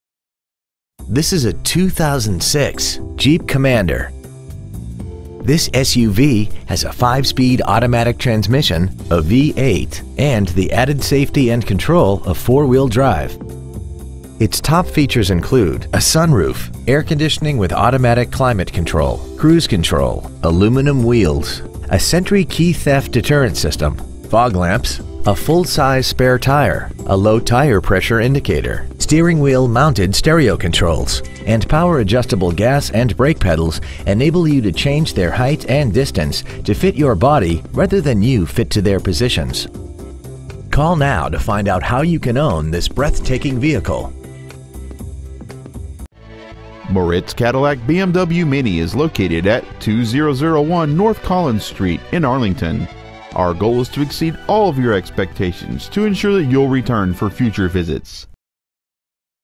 2006 Jeep Commander Arlington TX
Year : 2006

 Make : Jeep

 Model : Commander

 Engine : Gas V8 5.7/345

 Trans . : Automatic

 Exterior : Bright Silver Metallic

 Miles : 107,980

 Interior : Coming Soon

 2001 N. Collins St.

 Preowned 2006 Jeep Commander Arlington TX